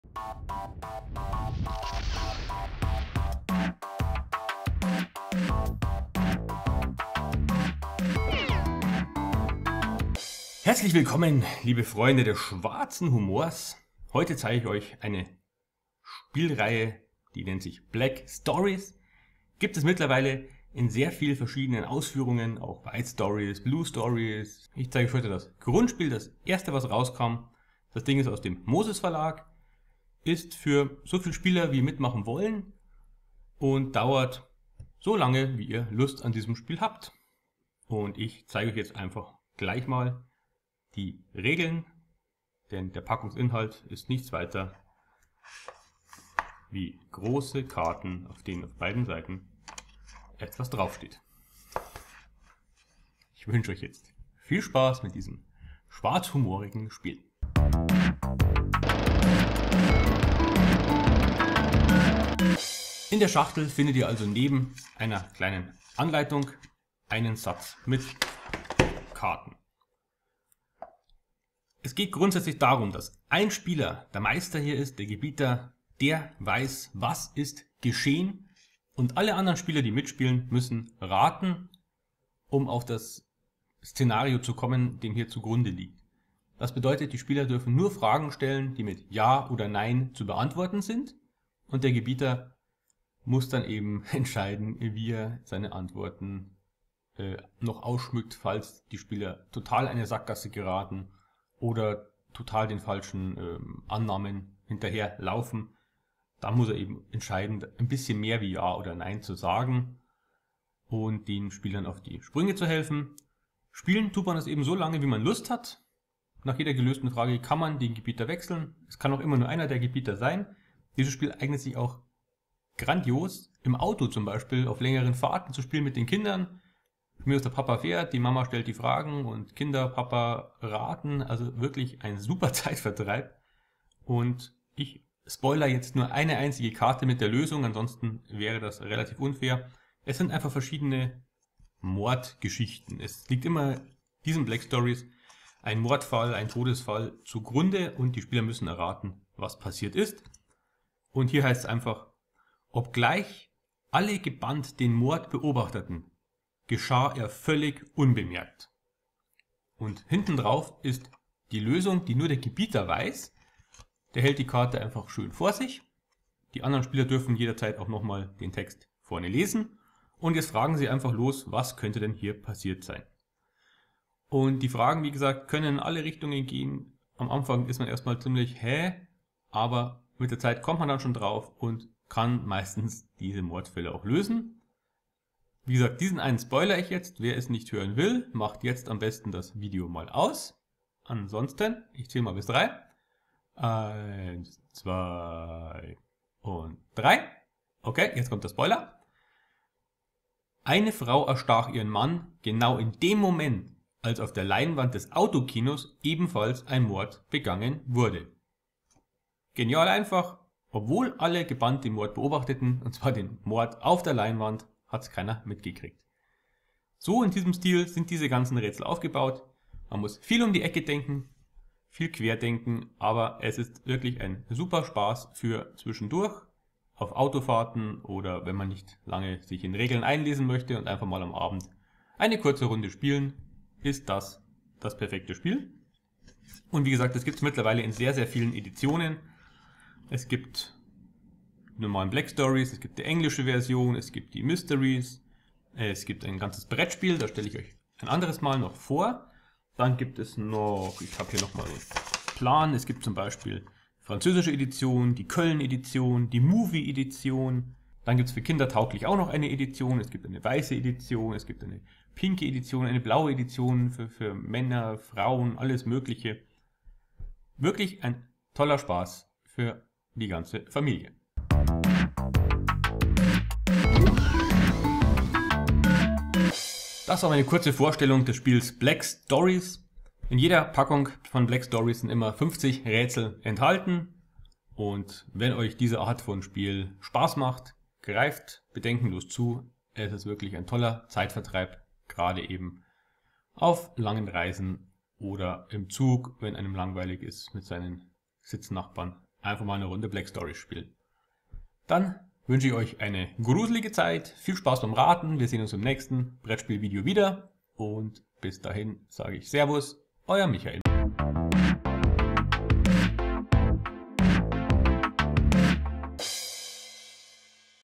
black stories | Verlag: moses.
► KEIN BRETTSPIELVIDEO MEHR VERPASSEN:
► DAS SPIEL BEI AMAZON KAUFEN:
► UM WAS GEHT ES BEI DIESEM SPIEL:
Rabenschwarze und mysteriöse Todesfälle stehen auf dem Programm. Was ist passiert? Nur der Gebieter weiß es. Schafft ihr es, durch einfach Ja/Nein Fragen das Geschehen aufzulösen?
► ZUSÄTZLICHE HINWEISE:
Das gezeigte Spielmaterial ist Eigentum des moses. Verlags.